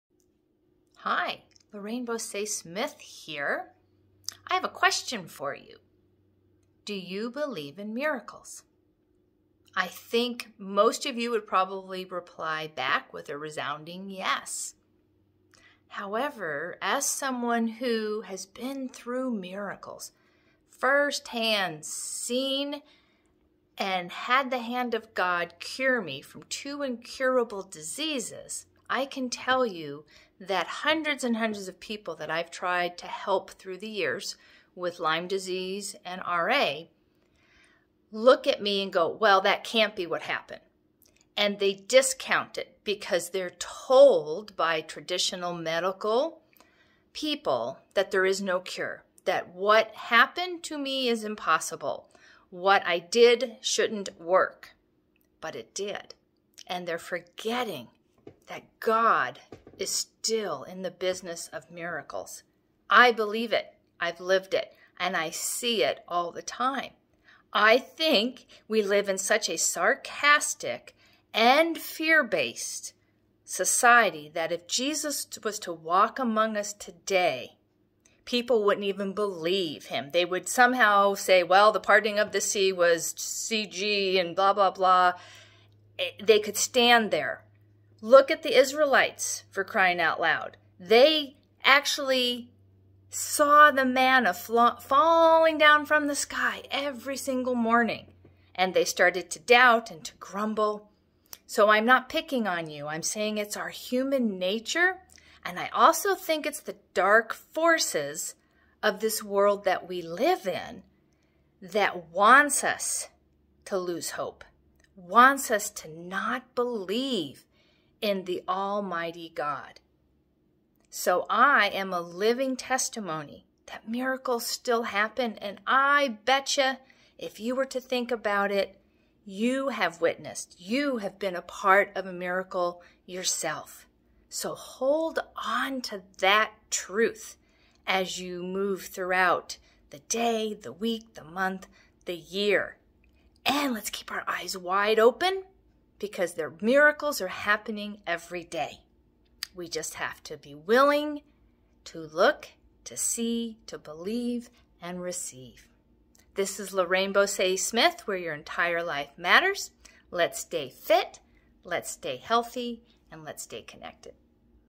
Do you believe in miracles
God is still in the miracle business if we open our hearts and eyes to see them! My life story is an example. I've beaten two incurable diseases. Learn how you can defy YOUR diagnosis in my book